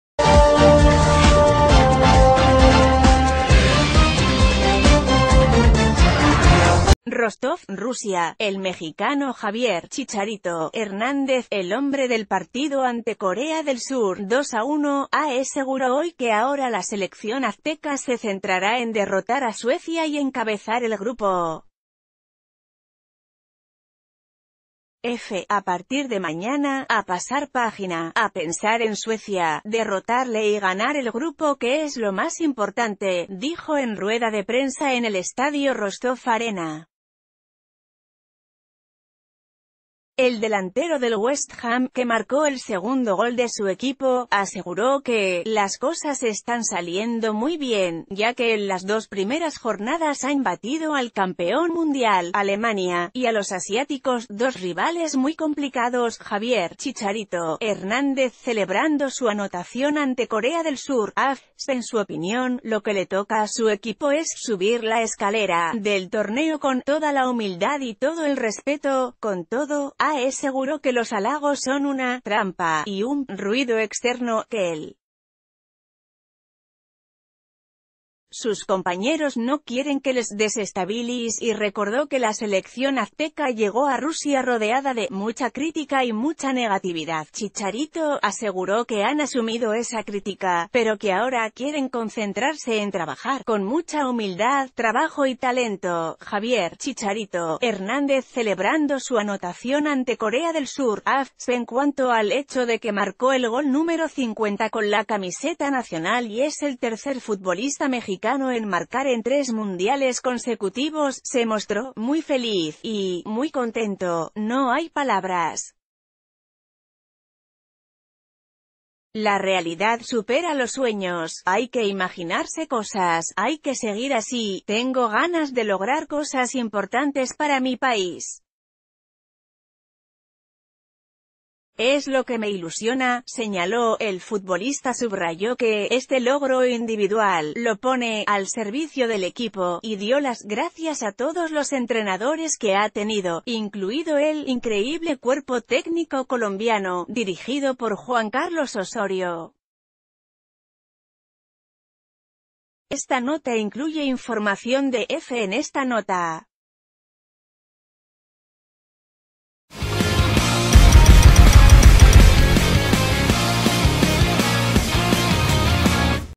Chicharito: "Ahora a derrotar a Suecia y a ganar el grupo"
Rostov, Rusia.- El mexicano Javier “Chicharito” Hernández, el hombre del partido ante Corea del Sur (2-1), aseguró hoy que ahora la selección azteca se centrará en derrotar a Suecia y encabezar el Grupo F.“A partir de mañana, a pasar página, a pensar en Suecia, derrotarle y ganar el grupo que es lo ...